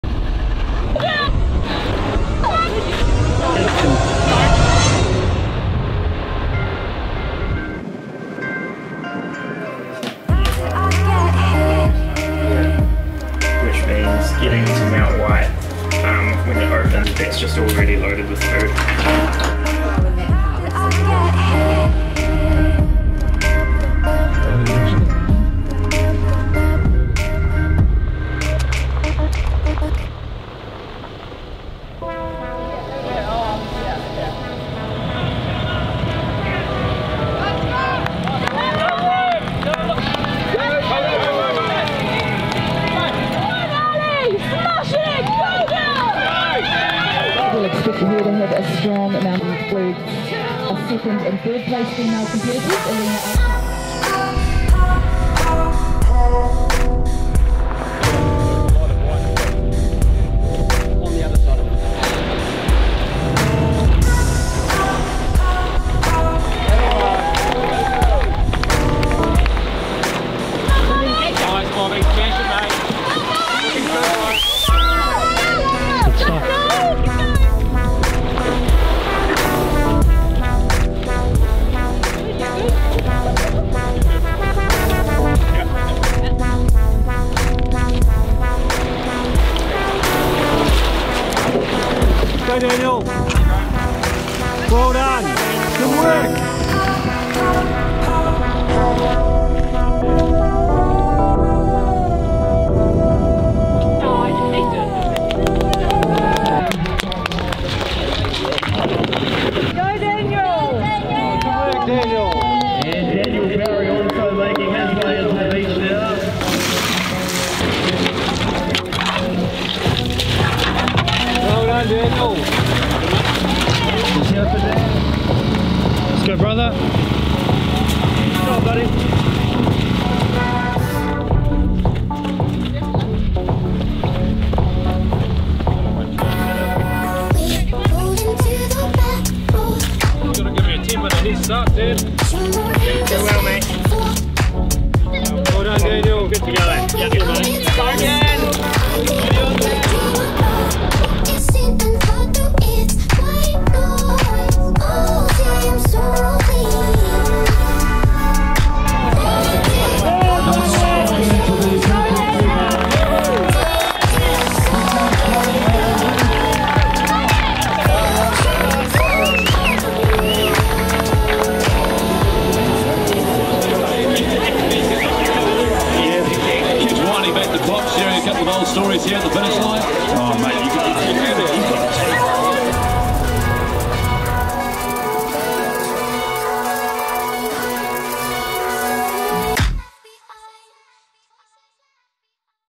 Coast to Coast 2020 - Daniel Barry
Following the bro in the 2020 Coast to Coast. He managed a 9th place finish which was super exciting!